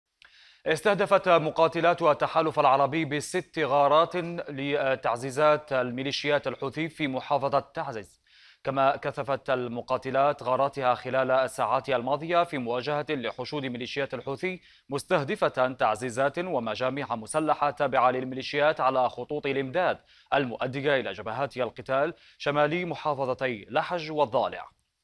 مقاتلات التحالف تدمر تعزيزات لميليشيات الحوثي
أخبار الآن | تعز - اليمن (وكالات)

استهدفت مقاتلات التحالف العربي بـ6 غارات تعزيزات لميليشيات الحوثي في محافظة تعز.

كما كثفت المقاتلات غاراتها خلال الساعات الماضية، في مواجهة لحشود ميليشيات الحوثي، مستهدفةً تعزيزات ومجاميع مسلحة تابعة للميليشيات على خطوط الإمداد المؤدية إلى جبهات القتال شمالي محافظتي لحج والضالع، وأدى القصف إلى تدمير عدد من الآليات وناقلات جند، وسقوط عشرات القتلى والجرحى في صفوف عناصر الميليشيات الانقلابية.

وقالت وزارة الدفاع اليمنية، إن الغارت استهدفت تعزيزات قتالية للحوثيين كانت في طريقها إلى مناطق المواجهات بجبهة كرش والقبيطة شمال محافظة لحج.

وأفادت مصادر محلية بأن مقاتلات التحالف دمرت أربع عربات عسكرية كانت تحمل مقاتلين تابعين للميليشيات وعربة تحمل مدفعا ومركبة قتالية من نوع "بي إن بي" غرب منطقة مريس شمال الضالع.

كما استهدفت مقاتلات التحالف تجمعاً آخر للميليشيات الحوثية جنوب مدينة دمت، كان عناصره يستعدون لخوض القتال ضد قوات الشرعية، ما أدى إلى مقتل أكثر من 20 حوثياً.

تأتي هذه الغارات بالتزامن مع معارك عنيفة تدور شمال وشرق منطقة مريس.